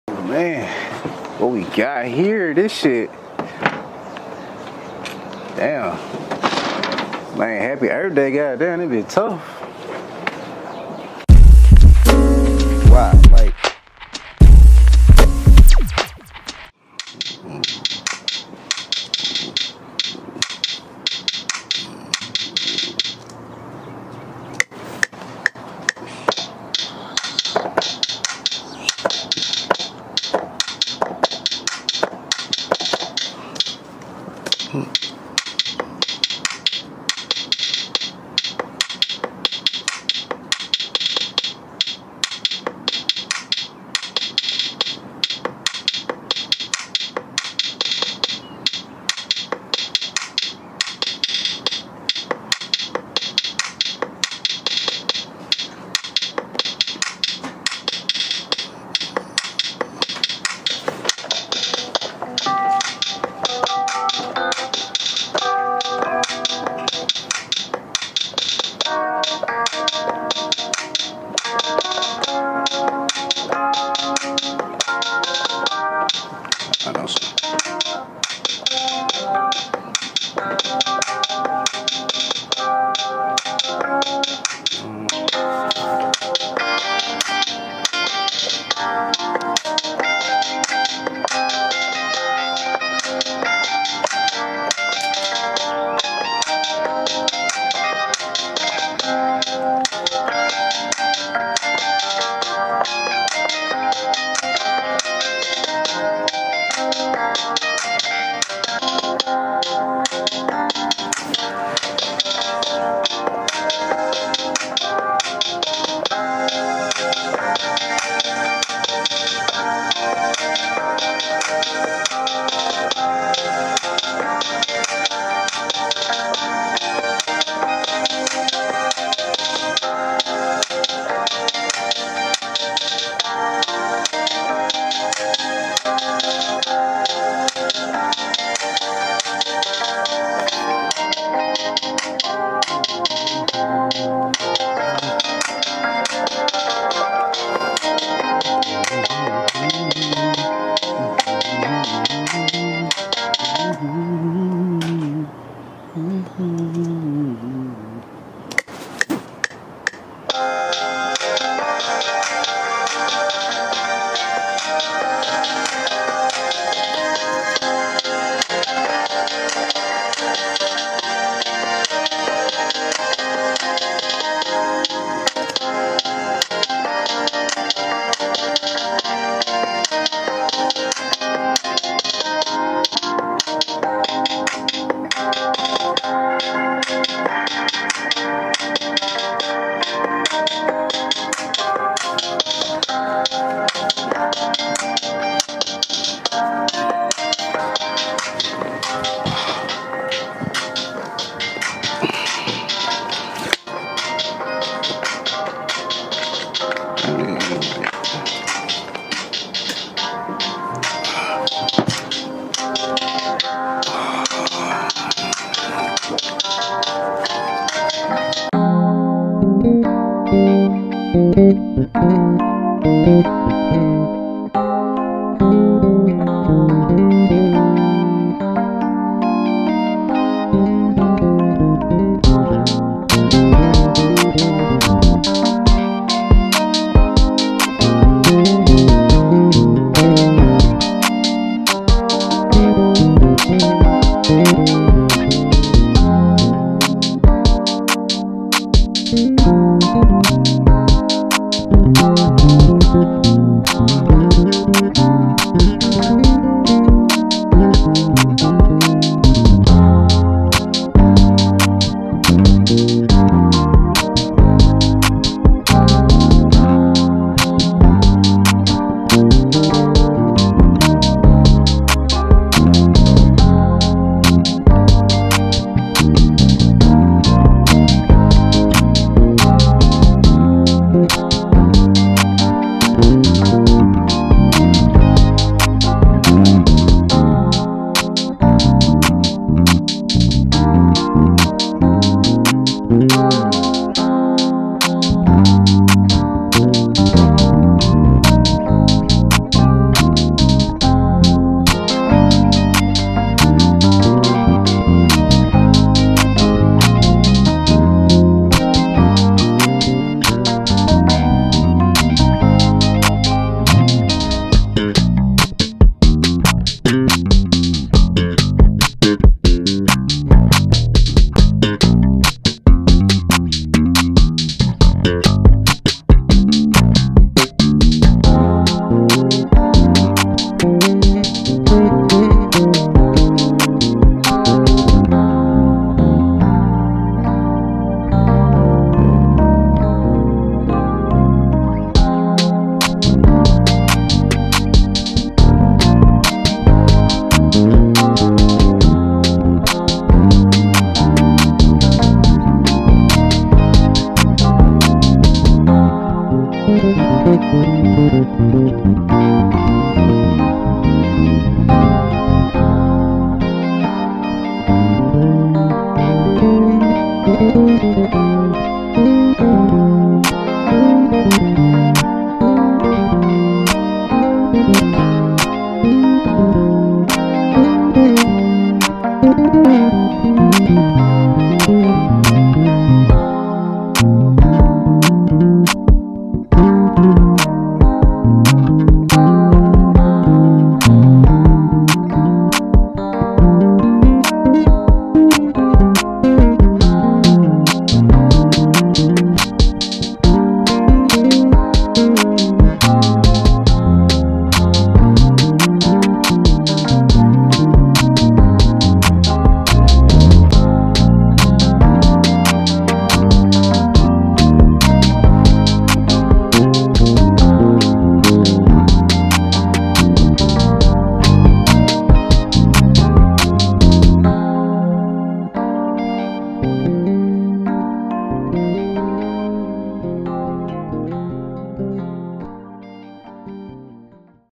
BCreativeTv: “Happy Earth Day” Episode 1 | Marcus “NezCARDE Reeves | New Vlog Series
Like to say I’m excited to start this entrepreneur life off with my first Vlog Series called “Creating with Creators”.🔥🔥🎥 In this series I’ll be documenting the dopest entrepreneurs (creators)in nashville creating inside the dopest spots in nashville EPISODE UNO 1️⃣WILL BE KICKED OFF WITH A COLD ASS MUSICIAN FROM NASHVILLE @nezcarde WHO TOOK HIS TALENTS TO ATL 🥶🎸 and of course for earthday WE HAD TO CREATE IN THE BEAUTIFUL @nashville.glass.house 🪴☀️‼️ One of the best spots in Nashville to shoot in if your a photographer 10/10 ratings FASHO !